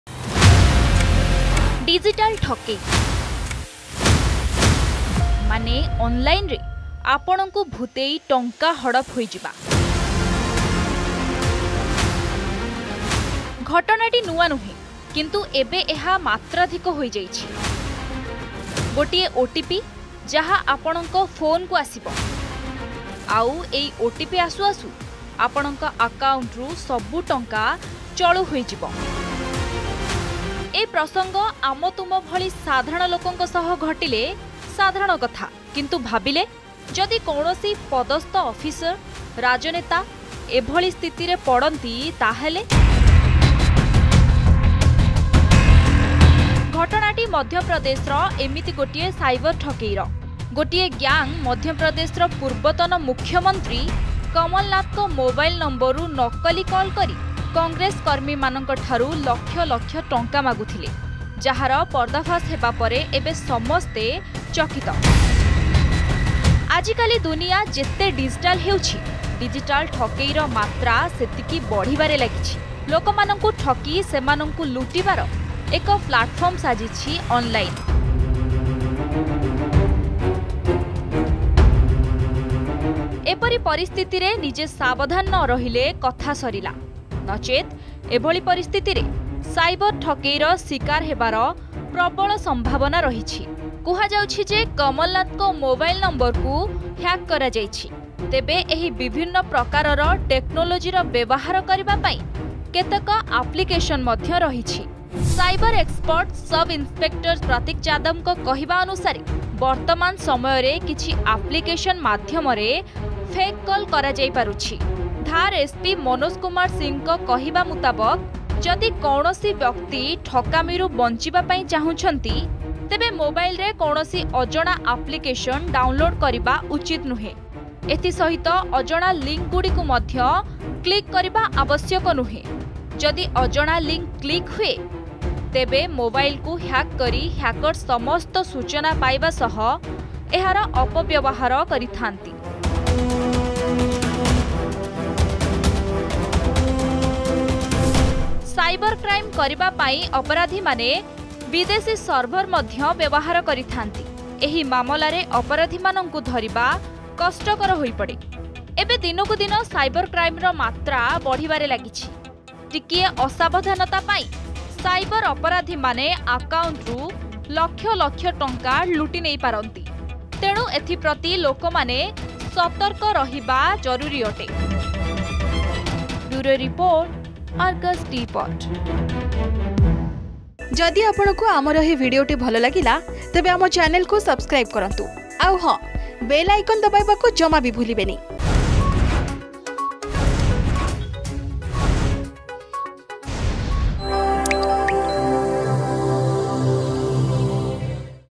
Cyber frauds through OTP involving CM
These days cyber crimes have been on the rise. Recently a cyber fraud involving ex CM of Madhya Pradesh, Kamal Nath has been doing the rounds.